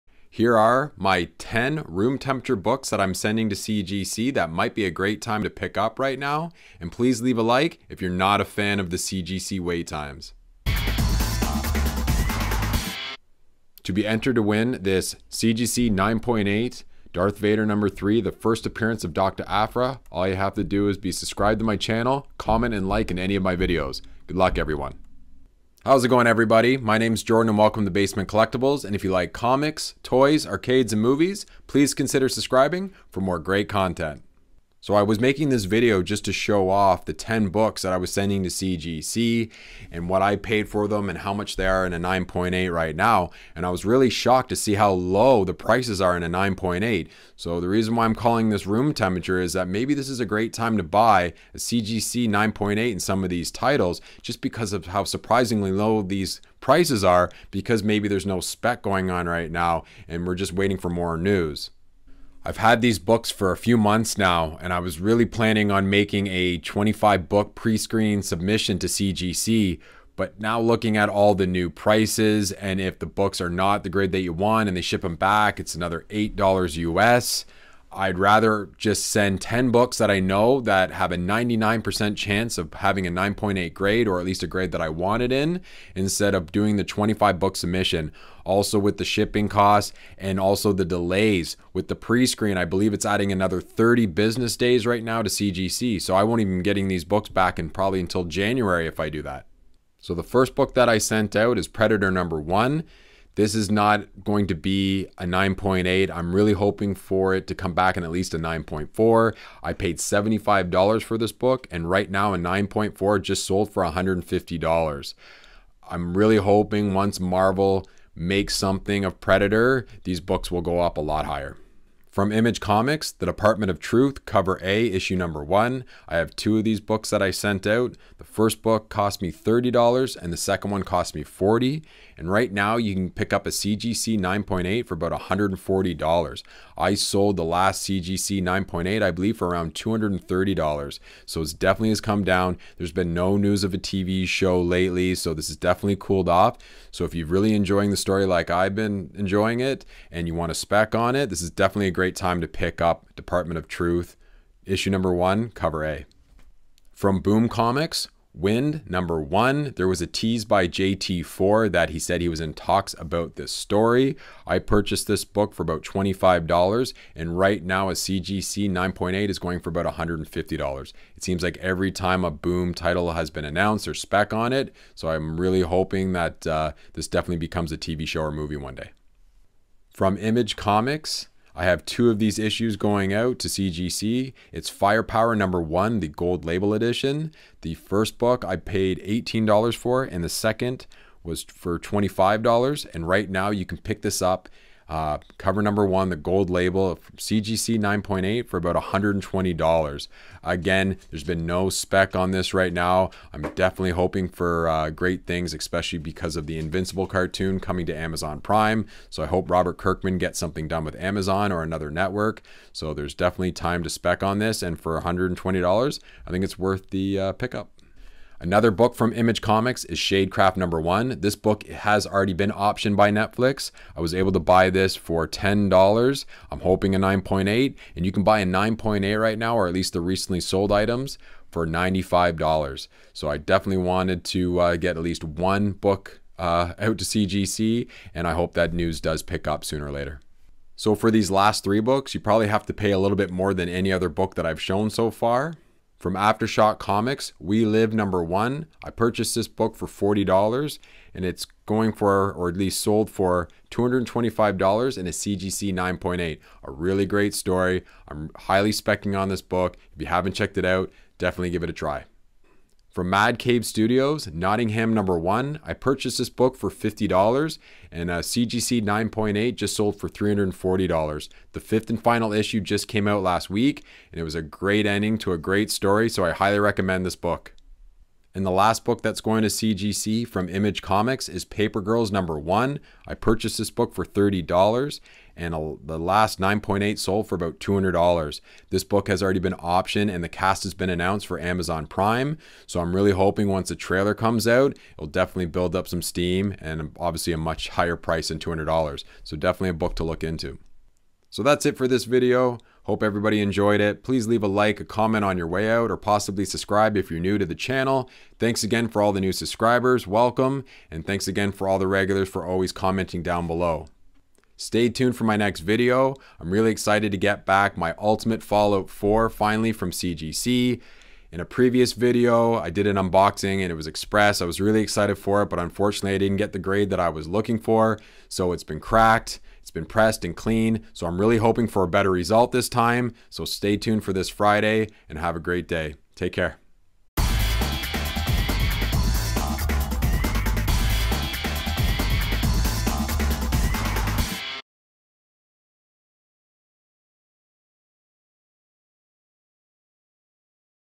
GREAT TIME TO BUY COMIC SPECS! CGC 10 BOOK SUBMISSION
In this video I go over my 10 CGC book submission and discovered some room temperature prices to take advantage of. Please let me know in the you're specing' on these books ?